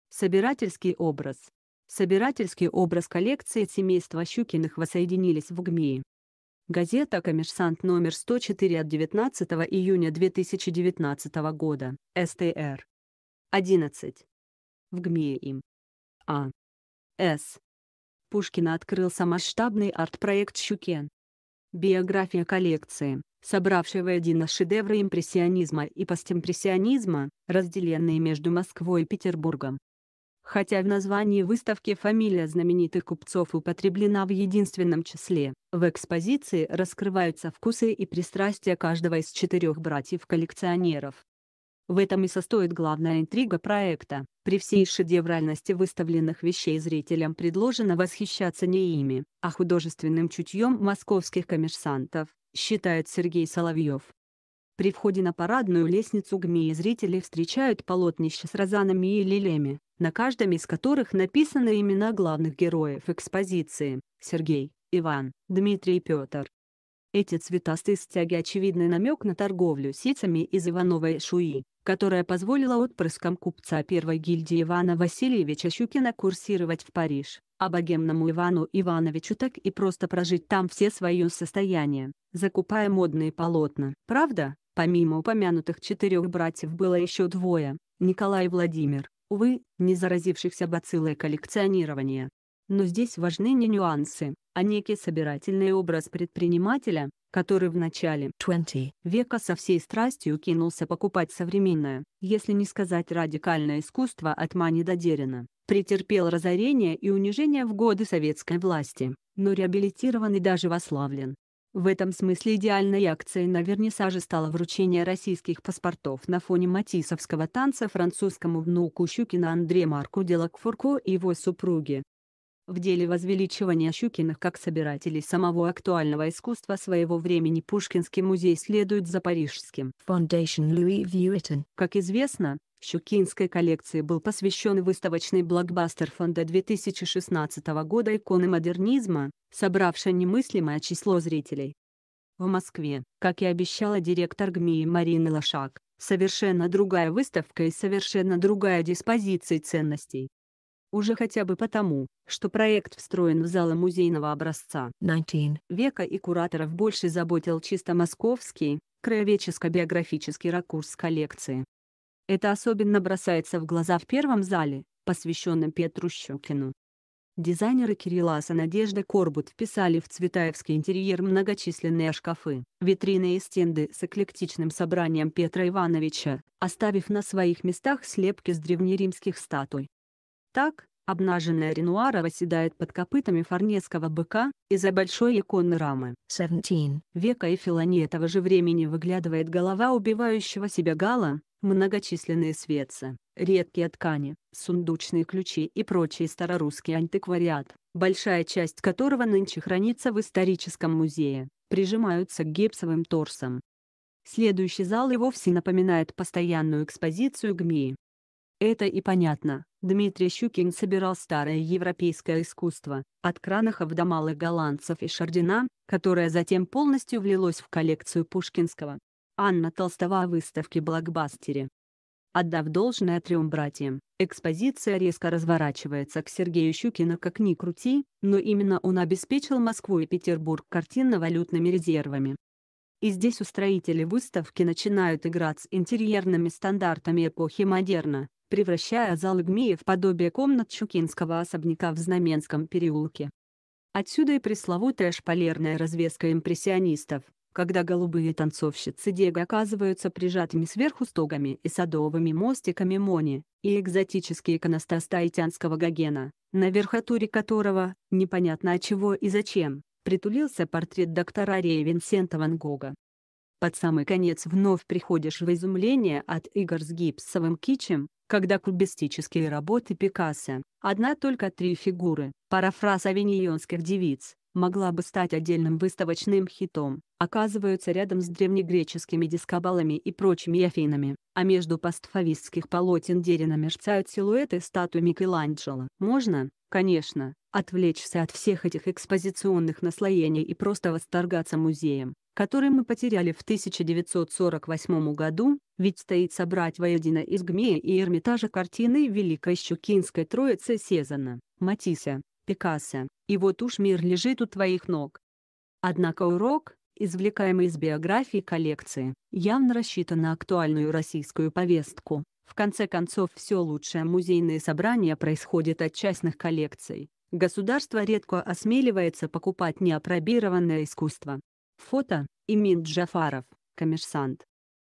Собирательский образ
Собирательский образ Собирательский образ Коллекции семейства Щукиных воссоединились в ГМИИ Газета Коммерсантъ 104 от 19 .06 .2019, стр . 11 В ГМИИ им . А . С . Пушкина открылся масштабный арт-проект Щукин . Биография коллекции , собравший воедино шедевры импрессионизма и постимпрессионизма, разделенные между Москвой и Петербургом . Хотя в названии выставки фамилия знаменитых купцов употреблена в единственном числе, в экспозиции раскрываются вкусы и пристрастия каждого из четырех братьев